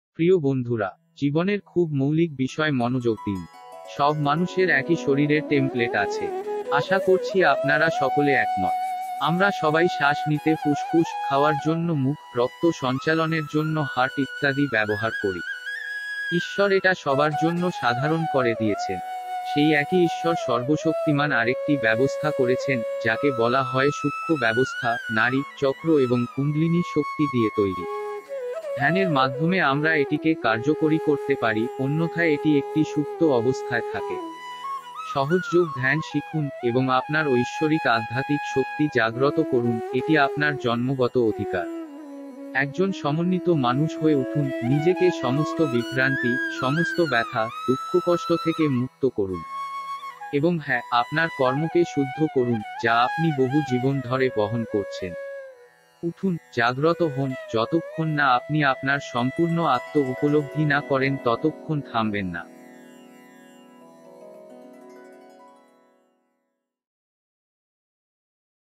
আত্ম-উপলব্ধির দিকে আপনার যাত্রা শুরু করুন!
প্রিয় বন্ধুরা,
জীবনের খুব মৌলিক বিষয় মনোযোগ দিন
সব মানুষের একই শরীরের টেমপ্লেট আছে - একমত?
আমরা সবাই শ্বাস নিতে ফুসফুস, খাওয়ার জন্য মুখ, রক্ত সঞ্চালনের জন্য হার্ট ইত্যাদি ব্যবহার করি।
ঈশ্বর এটা সবার জন্য সাধারণ করে দিয়েছেন!
সেই একই ঈশ্বর সর্বশক্তিমান আরেকটি ব্যবস্থা করেছেন - যাকে বলা হয় সূক্ষ্ম ব্যবস্থা, নাড়ি, চক্র এবং কুন্ডলিনী শক্তি দিয়ে তৈরি
ধ্যানের মাধ্যমে আমরা এটিকে কার্যকরী করতে পারি, অন্যথায় এটি একটি সুপ্ত অবস্থায় থাকে!
সহজ যোগ ধ্যান শিখুন, এবং আপনার ঐশ্বরিক আধ্যাত্মিক শক্তি জাগ্রত করুন - এটি আপনার জন্মগত অধিকার
একজন সমন্বিত মানুষ হয়ে উঠুন - নিজেকে সমস্ত বিভ্রান্তি, সমস্ত ব্যথা, দুঃখকষ্ট এবং দুঃখ থেকে মুক্ত করুন!
এবং হ্যাঁ, আপনার কর্মকে শুদ্ধ করুন - যা আপনি বহু জীবন ধরে বহন করছেন!
উঠুন, জাগ্রত হোন, যতক্ষণ না আপনি আপনার সম্পূর্ণ আত্ম উপলব্ধি না করেন ততক্ষণ থামবেন না!